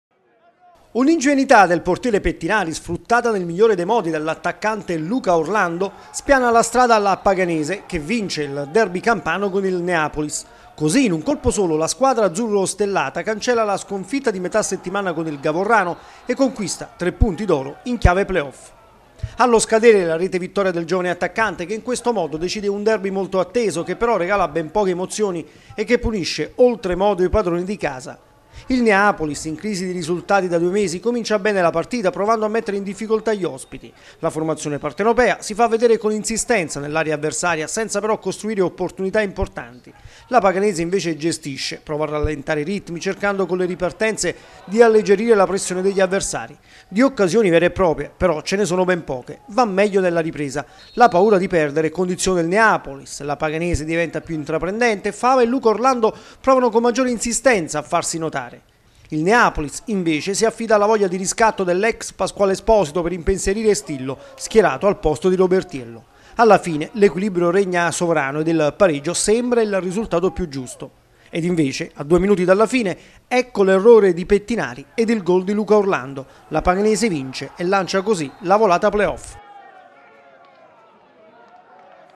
TG 120312 Paganese corsara con il Neapolis
Colpo esterno della Paganese che ha battuto in trasferta il Neapolis. Tre punti preziosi per restare in zona play off anche se l'Aquila ha vinto con il Giulianova mantenendosi a +4.